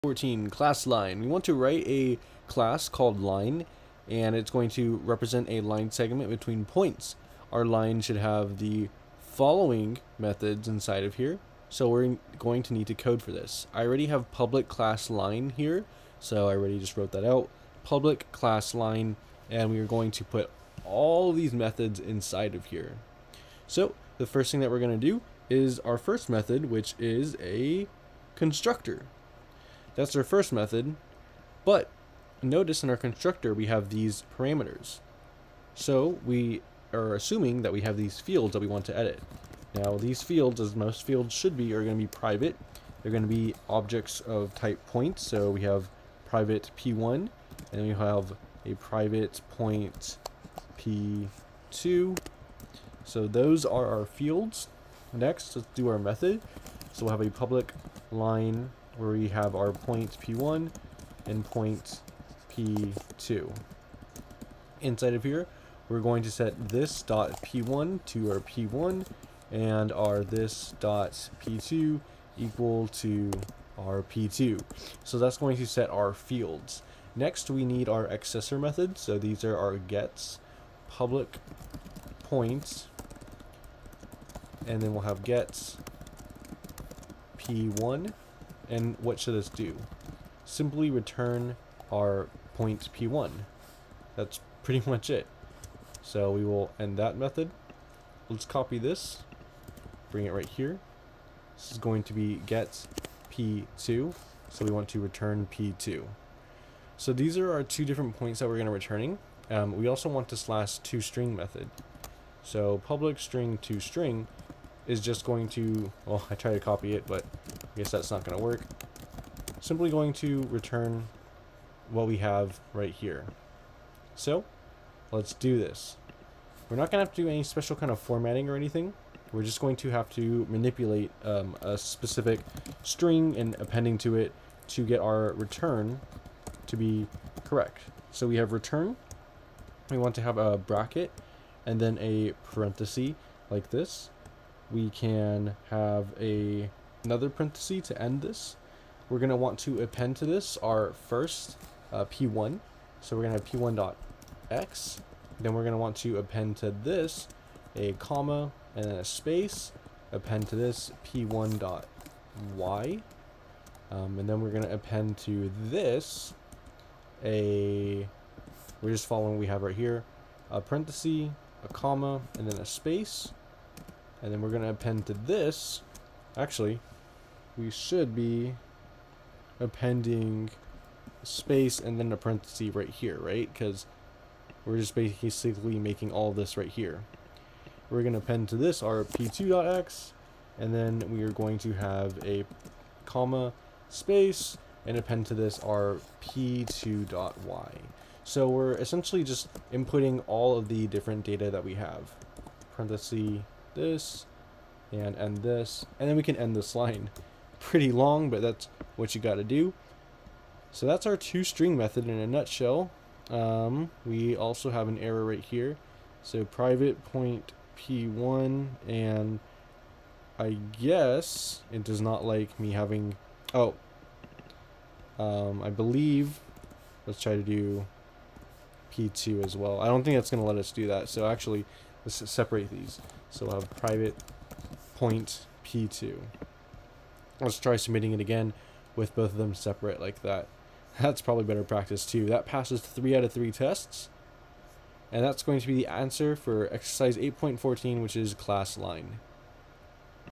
Java Practice It | Exercise 8.14 classLine | classes, constructors, fields, instance methods
Question:
Write a class called Line that represents a line segment between two Points. Your Line objects should have the following methods:

public Line(Point p1, Point p2)
Constructs a new line that contains the given two points.

public Point getP1()
Returns this line's first endpoint.

public Point getP2()
Returns this line's second endpoint.

public String toString()
Returns a string representation of this line, such as "[(22, 3), (4, 7)]".